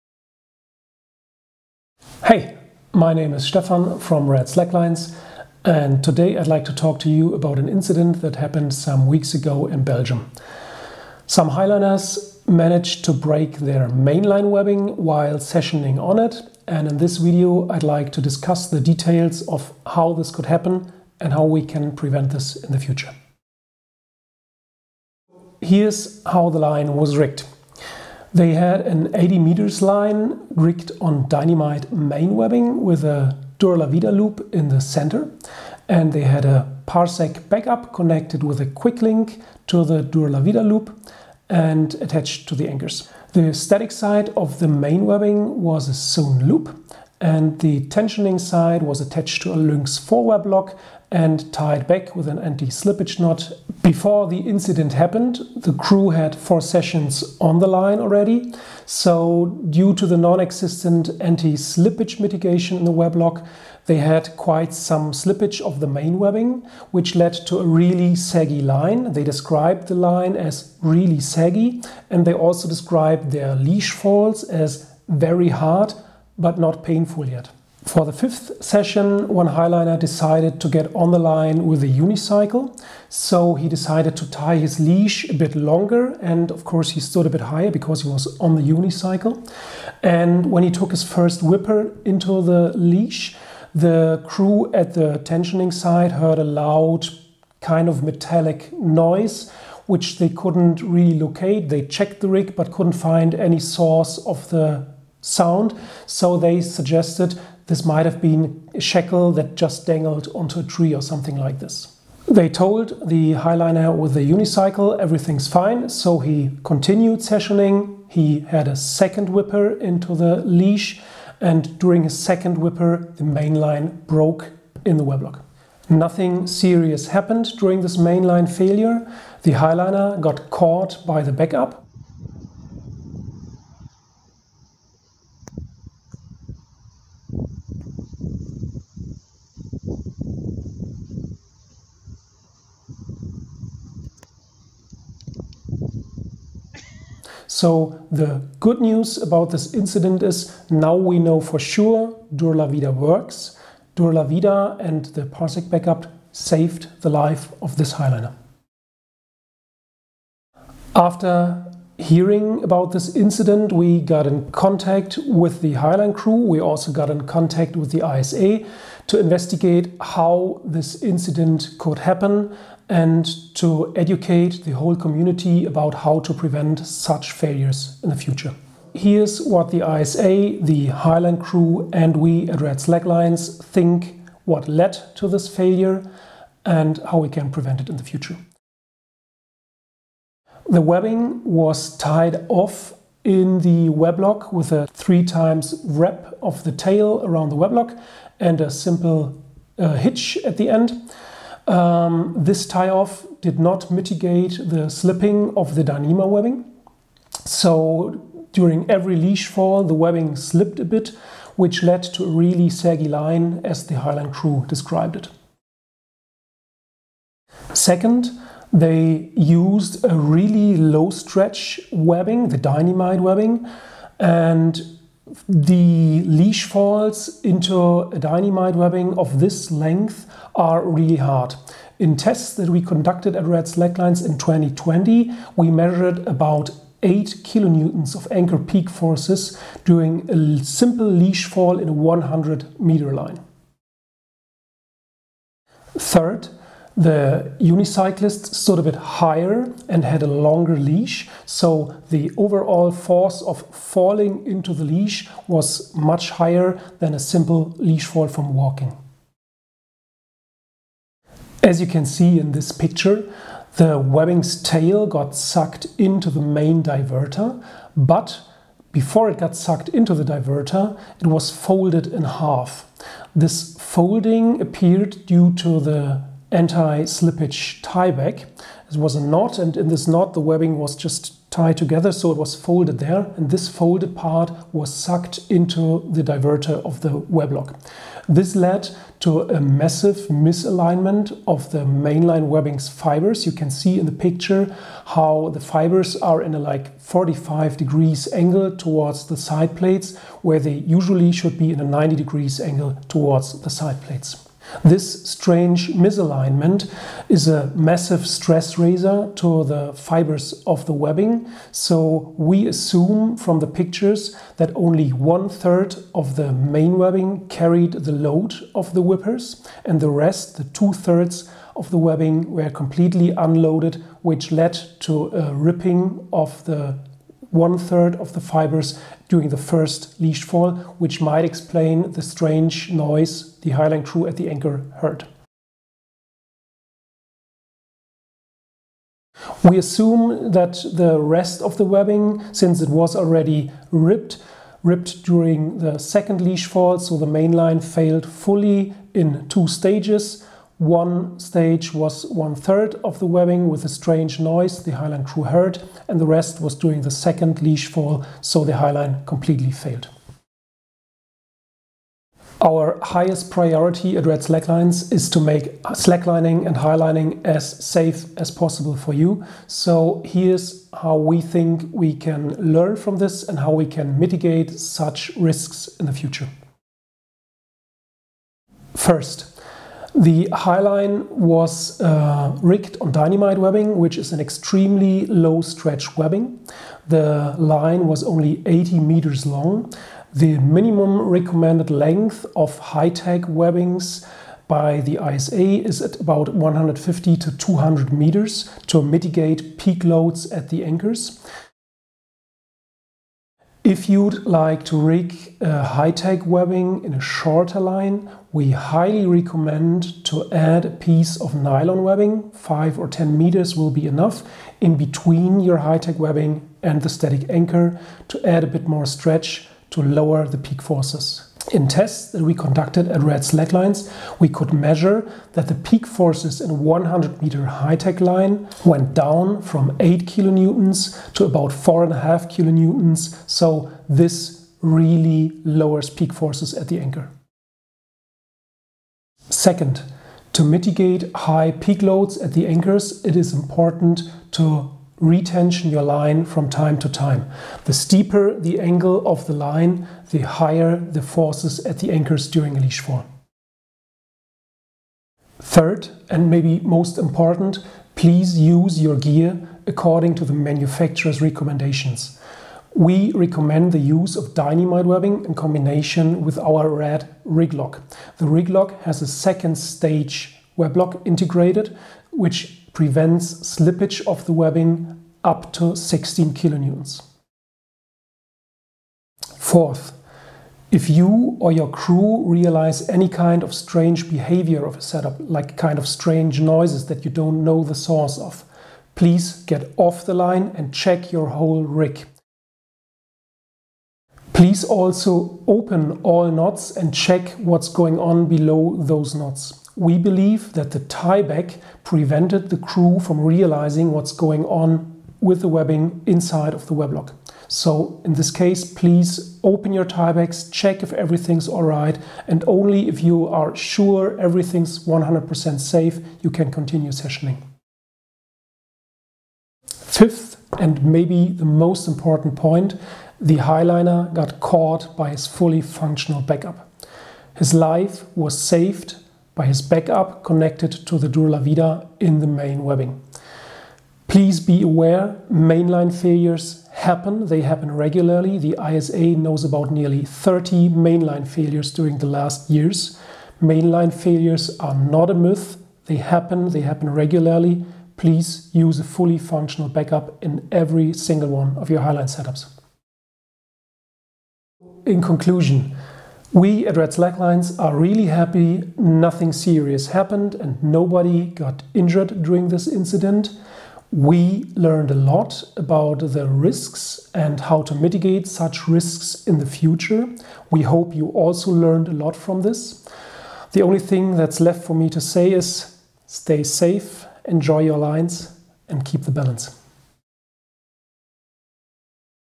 Highline Mainline Failure - contributing factors and risk management
A Highline Mainline Failure occured in Belgium. We highlight the contributing factors and try to educate on how to mitigate the risks in the future.
Please note: Highlining is inherently dangerous. It needs highly educated rigging skills and knowledge about material and the gear used.
This is neither a tutorial nor does this cover all relevant topics of highline rigging.